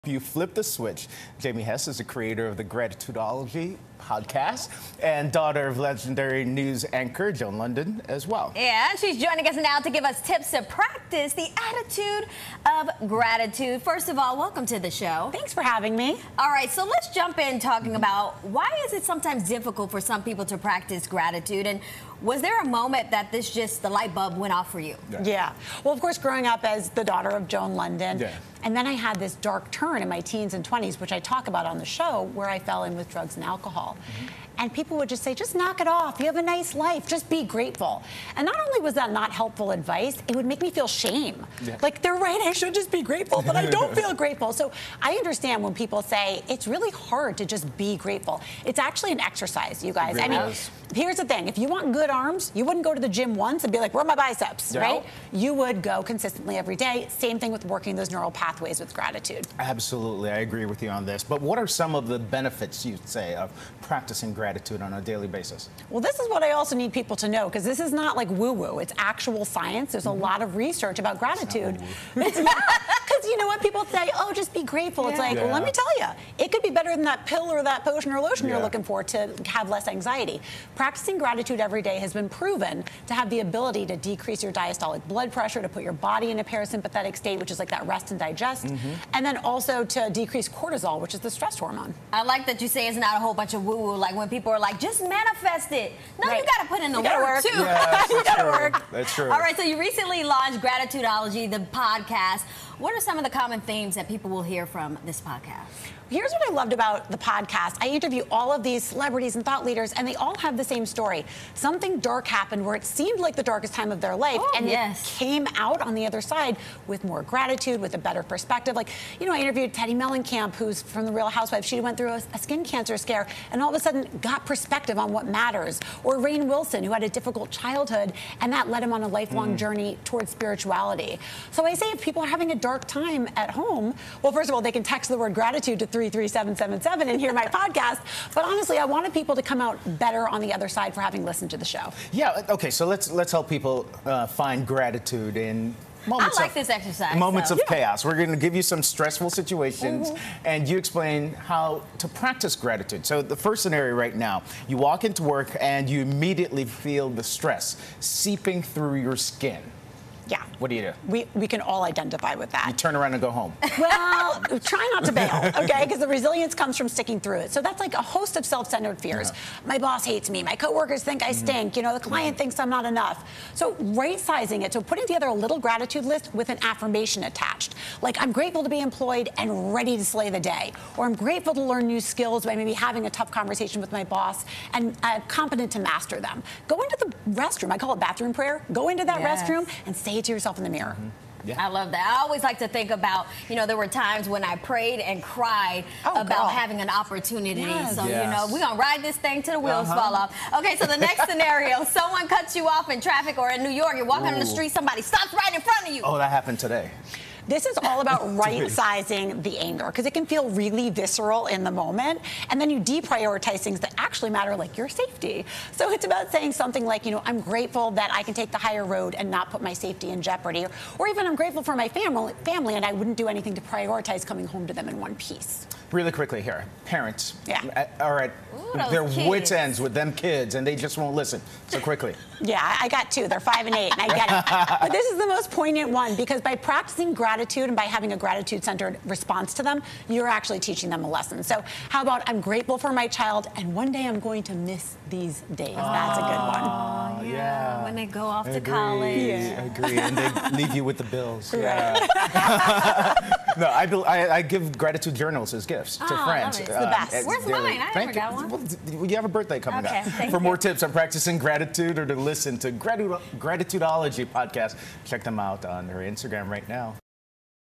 Pix11 | Gratitudeology | Jamie Hess
Topics Covered:

- Finding gratitude even when you don't feel like it's accessible
- Common themes on the Gratitudeology Podcast
- The power of gratitude
- Reframing negative thinking

1. Not everyone has an easy time practicing gratitude. What was your aha moment?

Just “getting grateful” doesn’t happen overnight. It is a practice, and takes exercise.

Very similar to how we will consistently exercise our biceps in the gym if we want to have great arms in a tank top, we have to exercise the neural pathways of our brain if we want to be intuitively grateful.

Neurons that fire together, wire together…

In other words, by integrating a (simple!) daily gratitude practice, you can rewire your brain to be less anxious and less focused on the negative and more focused on the positive.

2. What are some of the benefits of practicing gratitude? (please send a list)

- Doing a daily gratitude practice has been proven to:
- Decrease diastolic blood pressure
- Initiate the parasympathetic nervous system (the restful state)
- Decrease cortisol (the stress hormone)
- Improve sleep, mood, and immunity
- Decrease depression and improve productivity

3. You recently launched "The Gratitudeology" Podcast. What are some common themes you've come across?

The coolest thing I’ve come to realize is that, quite often, the darkest times in a person’s life can become their biggest pivot point to a life of happiness…. If they do the work to come out with a new sense of gratitude rather than a sense of resentment.

For instance, I spoke to Rainn Wilson (The Office), who’s difficult childhood put him on a lifelong journey to seek a sense of spirituality and gratitude.

Teddi Mellencamp (RHOBH) got a new perspective on life and what’s important by going through skin cancer.

Nev Schulman (Catfish) said he never would have learned his unique brand of empathy and gratitude for people’s stories if not for getting duped into a fake online relationship that thrust him into the spotlight and caused him to really look within at the purpose of it all.

Meditation & manifestation guru Gabby Bernstein said she never would have become the Oprah-endorsed spiritual leader, deemed “a new role model” by the New York Times… if not for her time as a drug addict.

My takeaway for audiences is this: you may feel like you’re in your darkest moment, but the light is just around the corner, and if you keep striving towards it, your greatest joy and enlightenment could follow.

4. Let's help people find gratitude in moments of chaos. We'll give you some stressful situations, and you explain how to practice gratitude.

SCENARIO #1:  You walk into work and you immediately feel the stress seeping through your skin?

In this scenario, we’re usually focused on a host of self-centered fears. “My boss thinks I suck, my co-workers hate me, the client thinks I’m stupid.” These thoughts only persist so intrusively because we continue to lean into them.

A simple reframe is to make a short gratitude list that includes affirmations:

- I’m grateful to be employed – and ready to take on the day.
- I’m grateful to have a chance to learn new skills today – and competent to master them.
- I’m grateful to be an active member to my team - and proud of the work I contribute.

SCENARIO #2:  Someone cuts you off in traffic and road rage hits?

This one is more of a “right-sizing” exercise. The anger towards the other person can be very visceral and real in the moment, but its importance is inflated (and we de-prioritize things that are actually important - like our safety).

The exercise here would look like this:

- I’m grateful for my ability to take the higher road and not react to this person.
- I’m grateful for my safety and wouldn’t do anything to jeopardize it.
I’m grateful for my family, and coming home to themin one piece  is my only priority.
- I’m grateful I can allow the other person to act however they want – my only job is to control myself and “keep my proverbial side of the street clean”

SCENARIO #3:  Parents who are at their wits end because their kids just won't listen?

This is one of the most challenging situation, but also the most poignant… because when our kids see us making a good choice grounded in gratitude, they absorb that life skill.

- I am grateful that I can remain calm and demonstrate emotional resilience for my child
- I am grateful that I have done the work to not overreact… because whenever I fly off the handle, I regret setting a bad example with my words or actions.
- I am grateful my child is safe. I don’t like their choice, but I can accept it as age-appropriate… and one day I’ll miss these moments. (great awareness!)